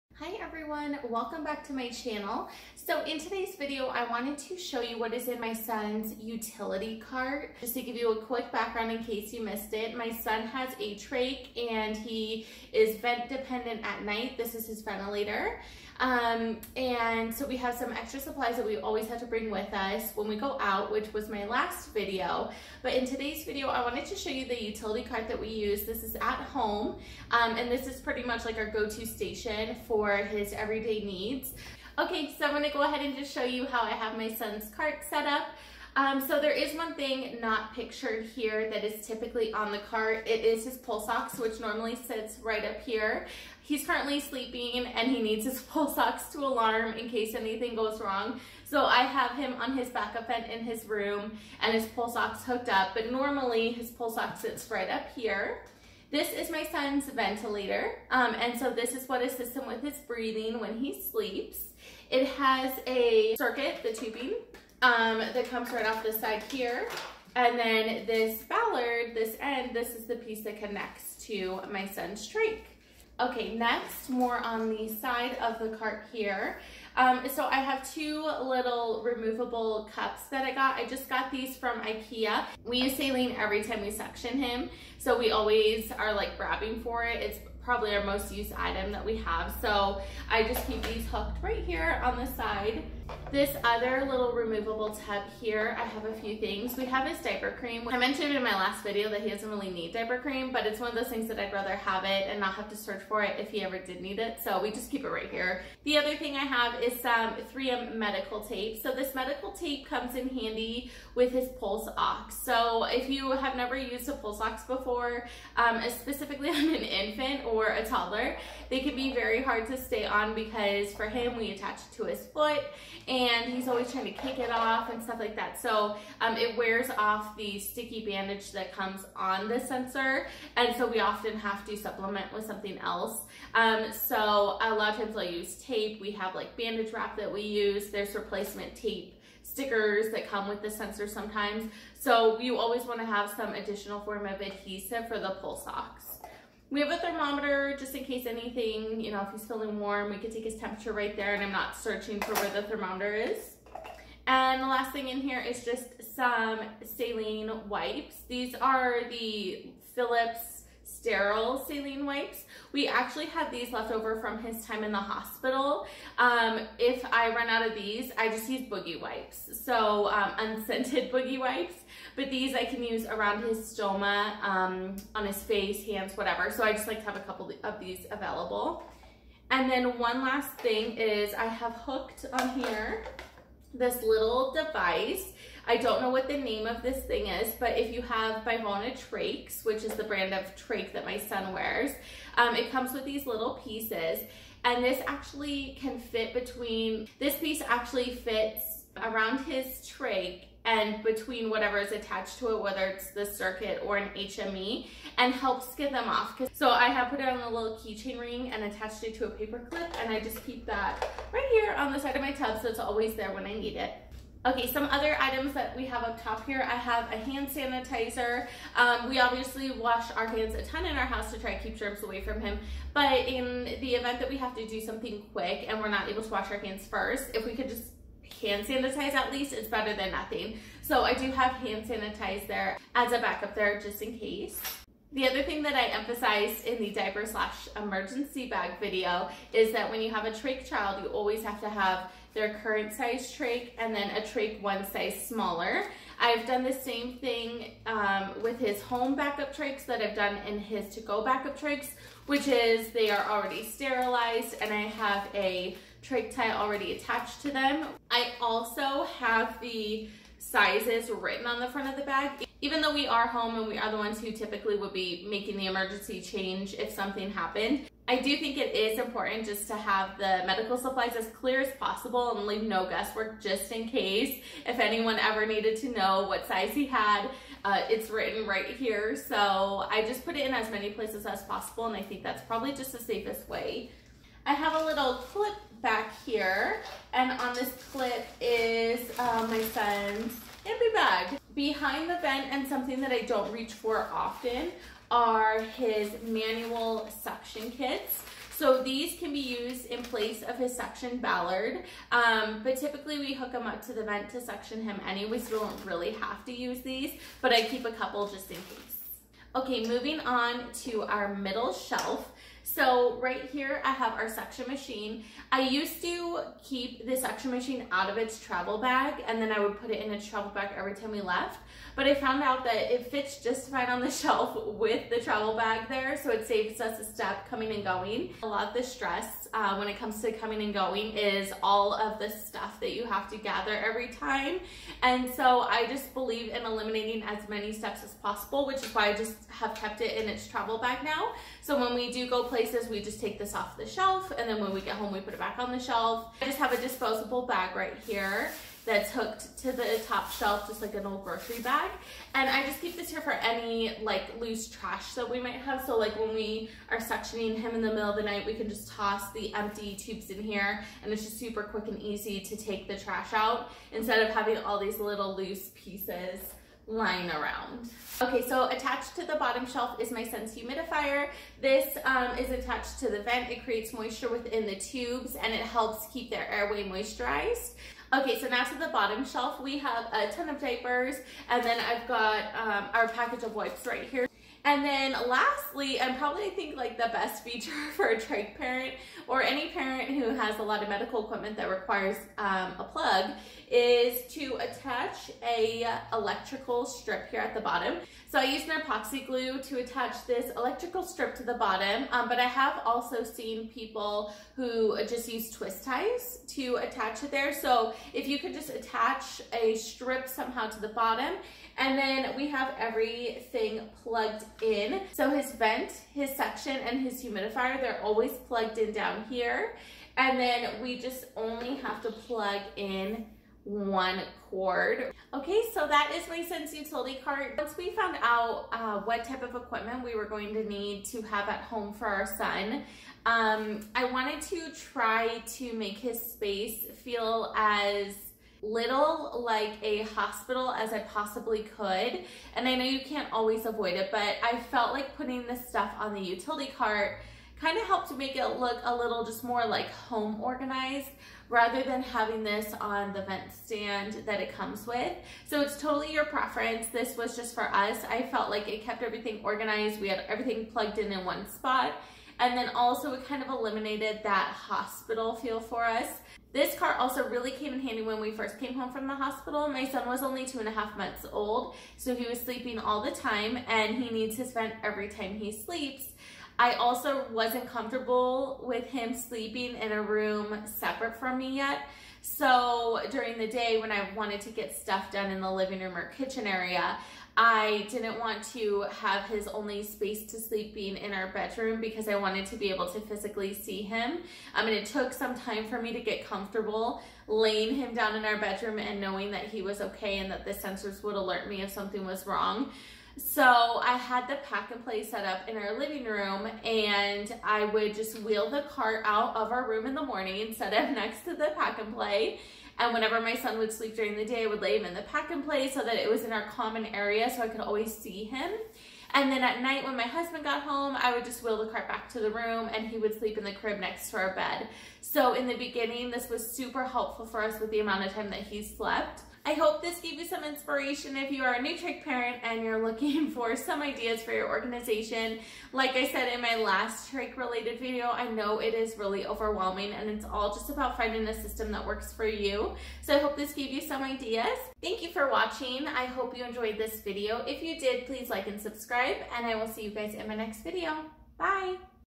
Utility Cart Organization | Baby Supplies and Medical Equipment
In this video I share with you how I have organized my son's medical equipment and baby supplies onto a 3-tier utility cart. This has been a game changer!